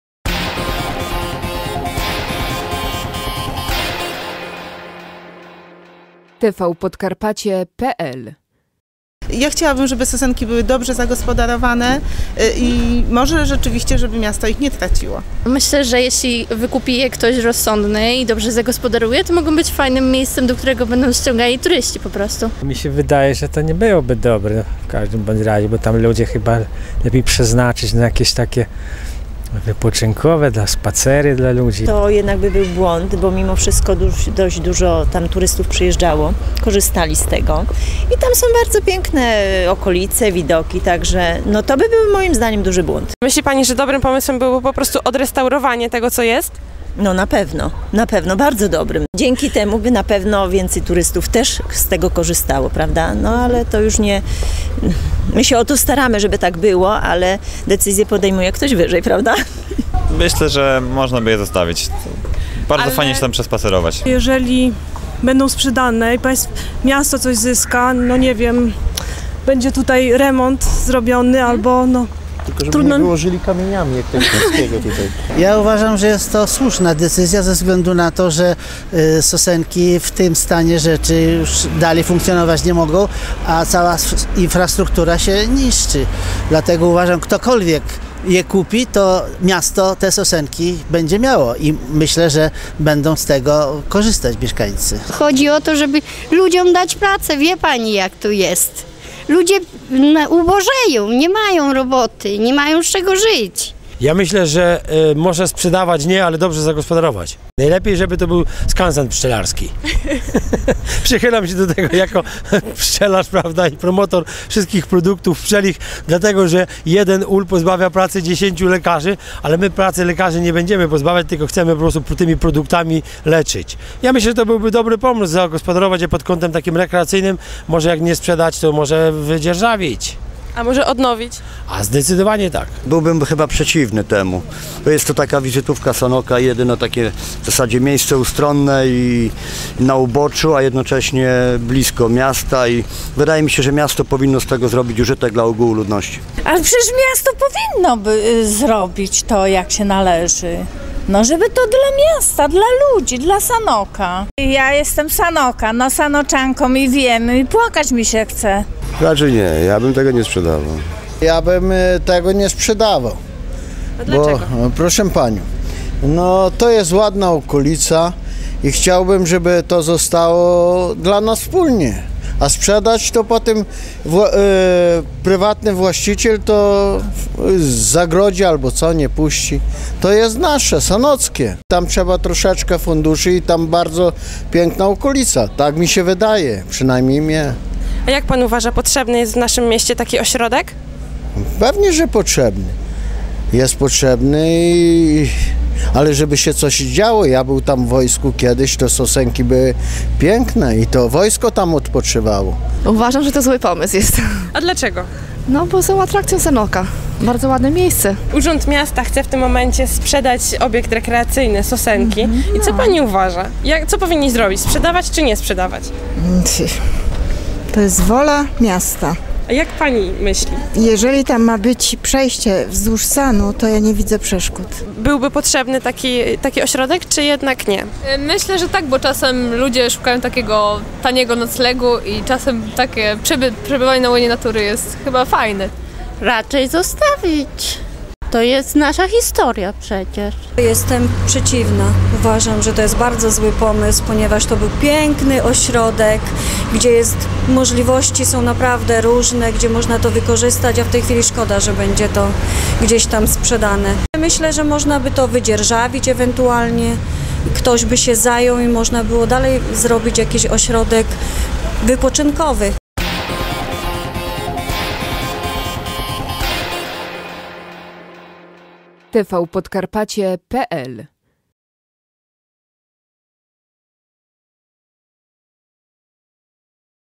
Sprzedawać Sosenki czy nie? Opinie mieszkańców (FILM, SONDA)
SANOK / PODKARPACIE. Rada Miasta Sanoka zgodziła się na sprzedaż ośrodka wypoczynkowo-rekreacyjnego Sosenki. Czy jest to dobry pomysł? Wydaje się, że podobnie jak radni, podzieleni są mieszkańcy miasta. Więcej w materiale filmowym.